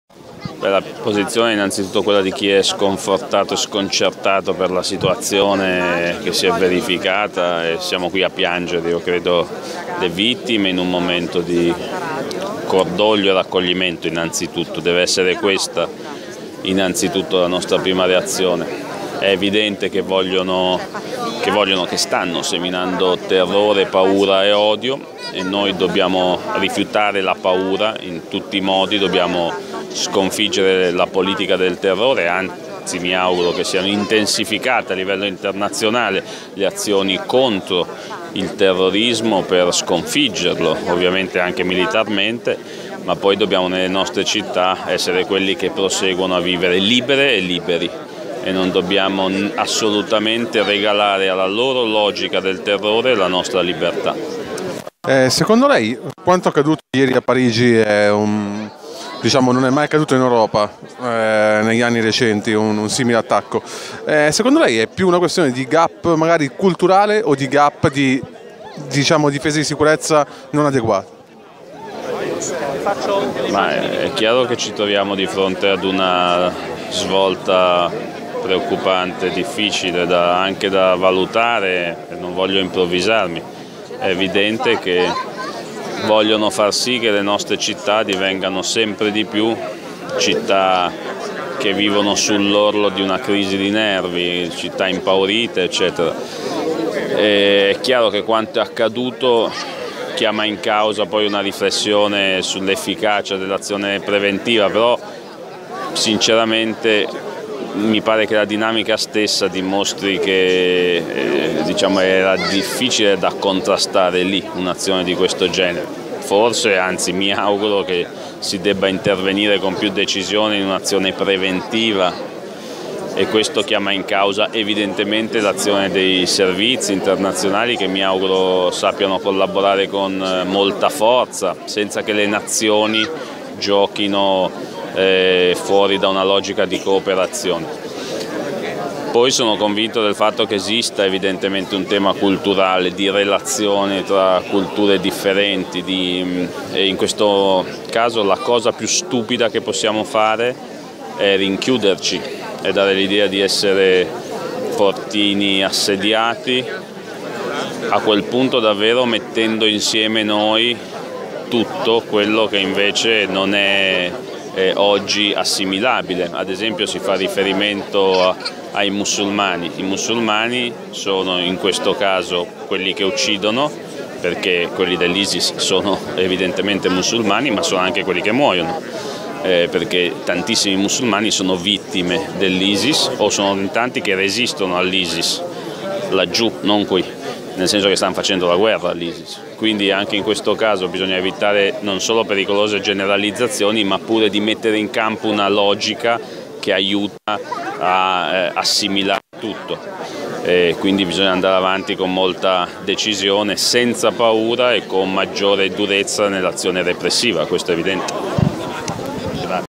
Majorino: “I musulmani sono quelli che uccidono e quelli che muoiono”
POLITICA (Milano). Questo pomeriggio in Piazza Fontana a Milano è stata organizzata una manifestazione per ricordare le vittime degli attentati di Parigi. L'assessore Majorino ha voluto partecipare parlando di quanto accaduto. "Sono sconcertato e siamo qui a piangere le vittime che hanno subito l'attacco. È evidente che stanno seminando paura, terrore e odio. Bisogna intensificare le azioni contro il terrorismo per sconfiggerlo anche militarmente. Ma poi nelle nostre città dobbiamo vivere liberamente". Chiude poi aggiungendo le sue preoccupazioni."Siamo di fronte a una svolta preoccupante ed è evidente che vogliono che le nostre città siano sempre in tensione e impaurite. Mi auguro che venga rinforzata la funzione preventiva per impedire altri attentati. I musulmani sono quelli che uccidono ma sono anche quelli che muoiono. In tanti di loro resistono all'Isis". (Francesco Marotta/alaNEWS)